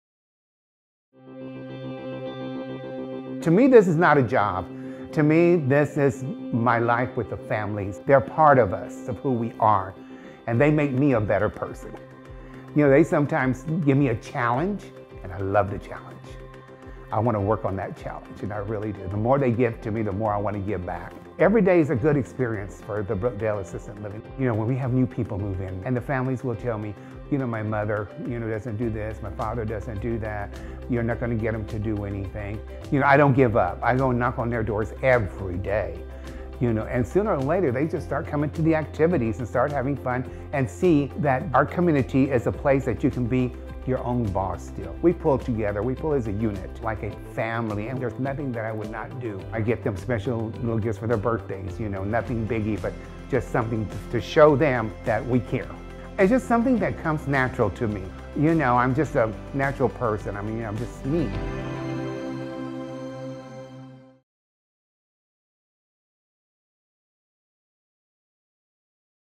More than Just a Job, We're a Family
To Ben Culler, his job is not just a job. It is his life and the residents are his extended family. He strives to go out of his way to help the residents get involved in activities and try new things. He doesn't back down from a challenge and he sees the potential in every person.